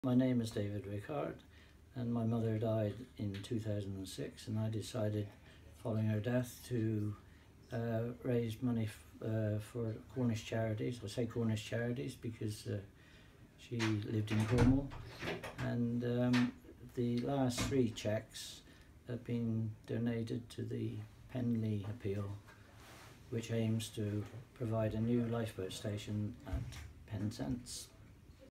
Pensioner With Paper Round Donates Wages To Cornish Lifeboat
He's raised £4,000 for Penlee's new station